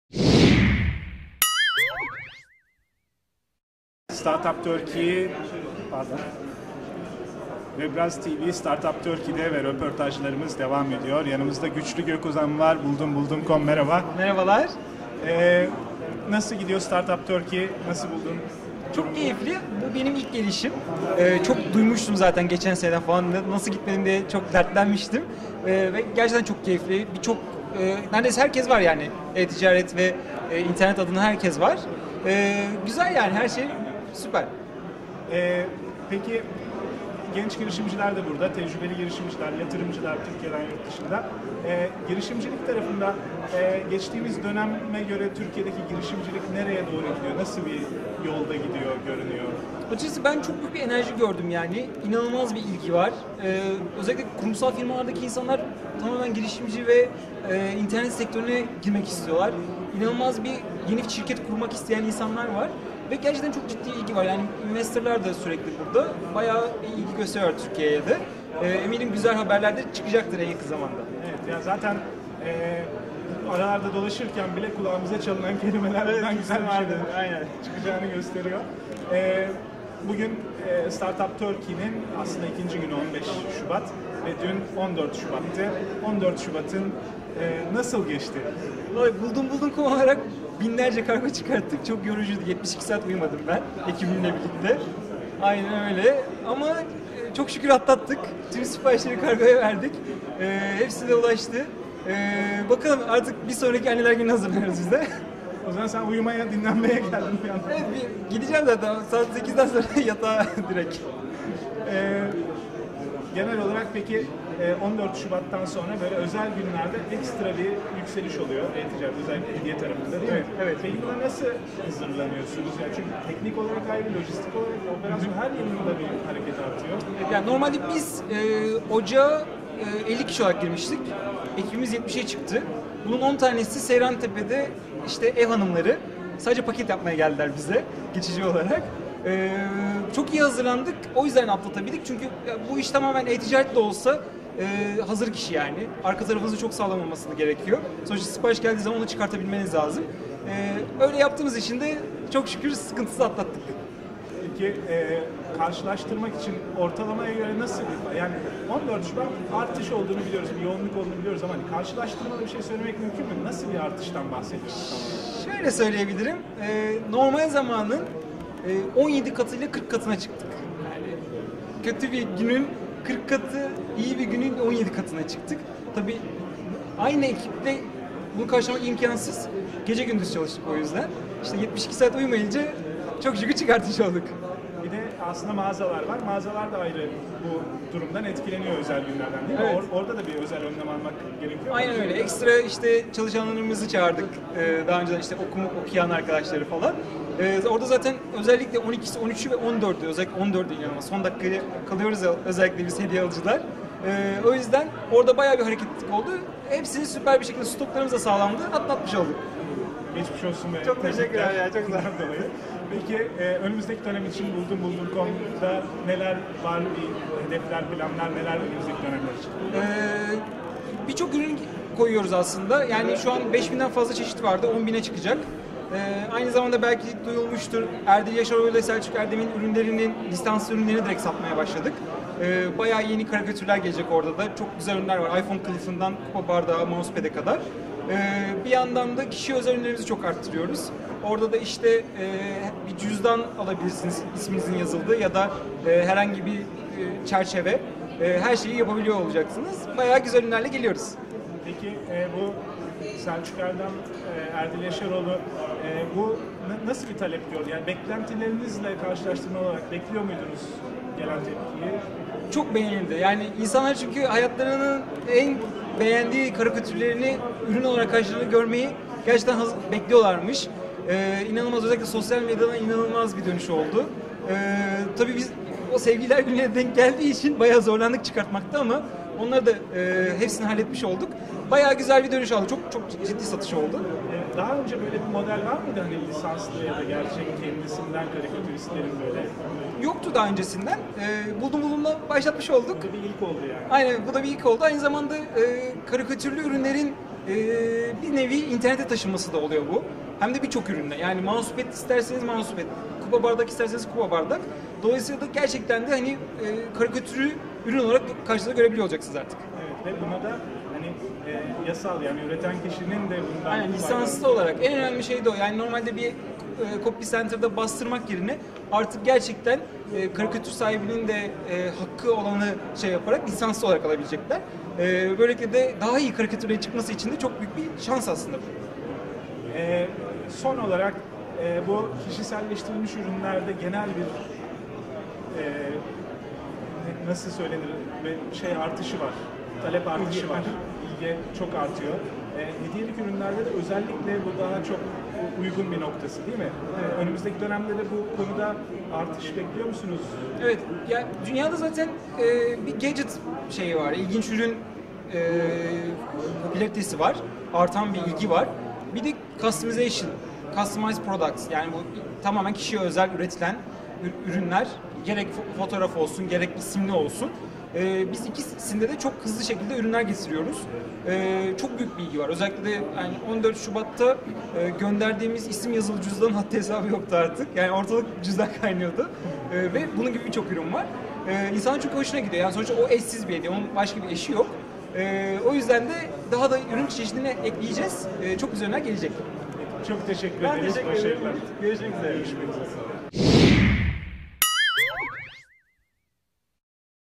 Startup Turkey 2013 röportajları: Güçlü Gökozan
Antalya'da düzenlenen Startup Turkey etkinliğinden yaptığımız röportajlarla sizlerleyiz.